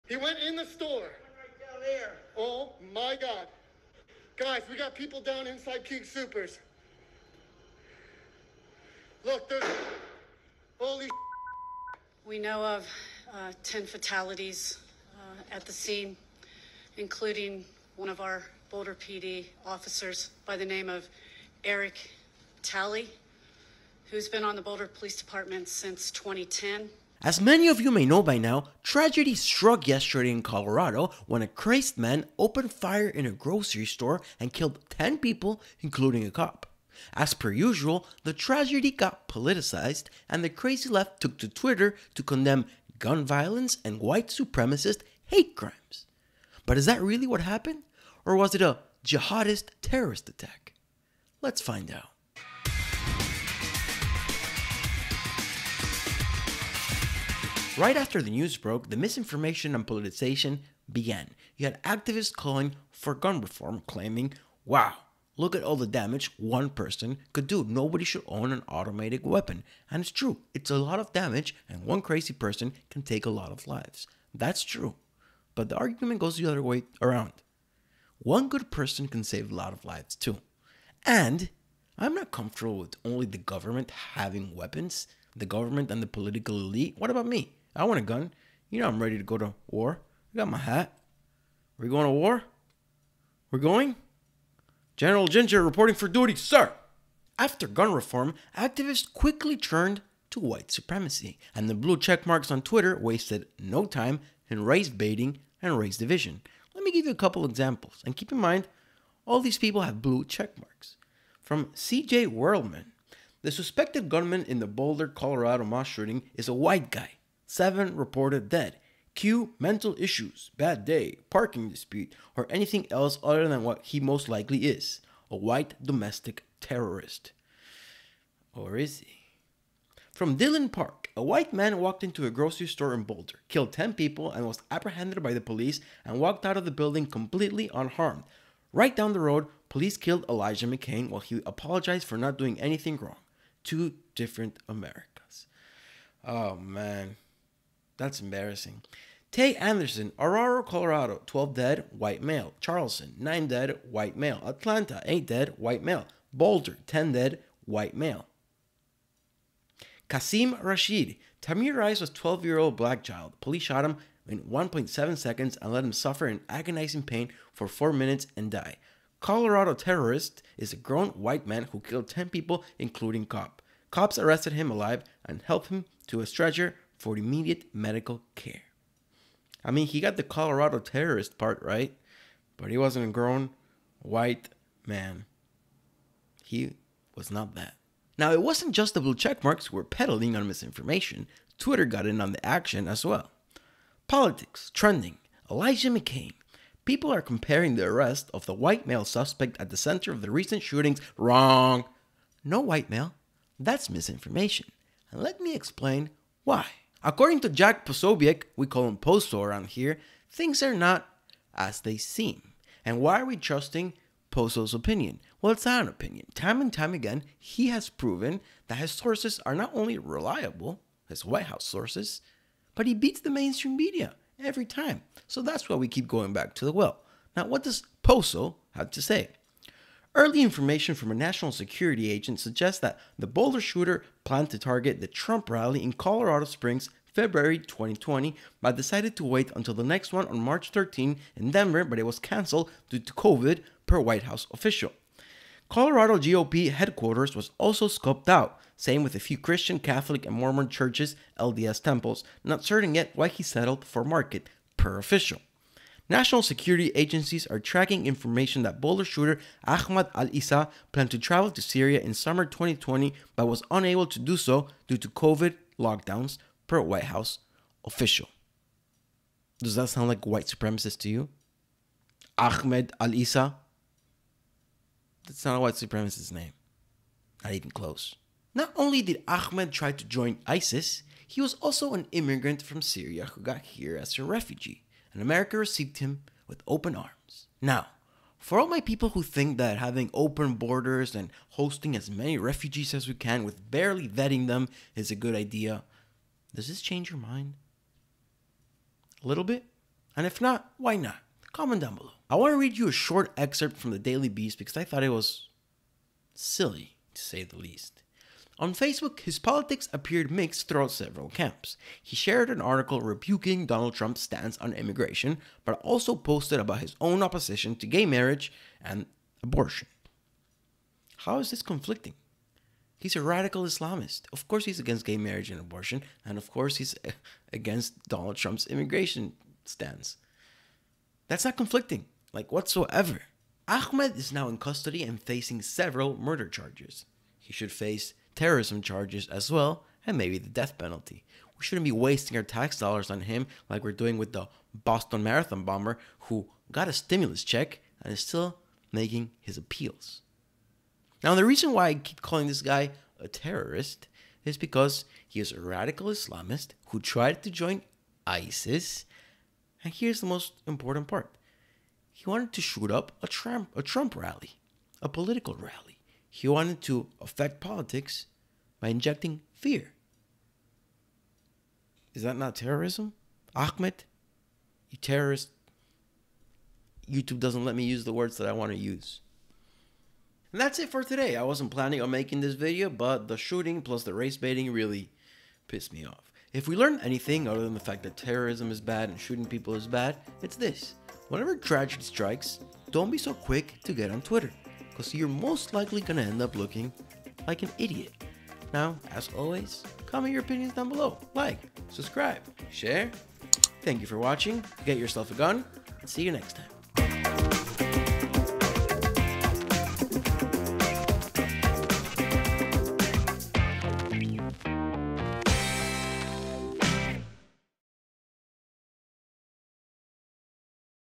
Colorado Shooting: 10 Lives Lost At The Hands Of A Radical Islamist Leftist
Tragedy struck Colorado when a mad man opened fire in a grocery store. Ten lives were lost including a police officer. The suspect is now in custody and charged with ten counts of murder.
The suspect, Ahmad Allissa, is a Syrian refugee who tried going back to Syria in order to join the ranks of ISIS. Should this be considered a terrorist attack?